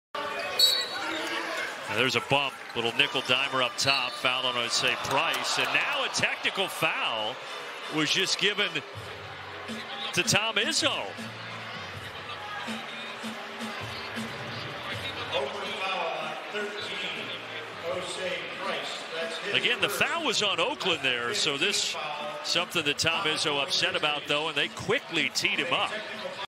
Tom Izzo gets T'd up while wearing an elf sweater 🎄
Michigan State Spartans head coach Tom Izzo argues a foul call and quickly is assessed a technical foul while wearing a festive sweater.